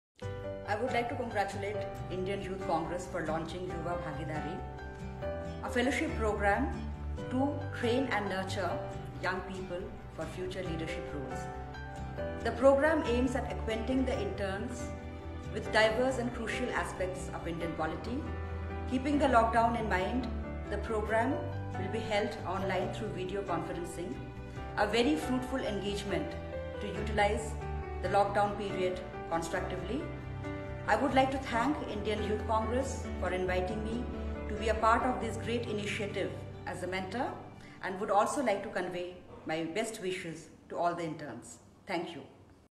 Smt Sharmistha Mukherjee , National Spokesperson, AICC and President, Delhi Mahila Congress explains
Smt Sharmistha Mukherjee , National Spokesperson, AICC and President, Delhi Mahila Congress explains the relevance of “Yuva Bhagidaari” during these tough days and conveys her wishes towards it’s success.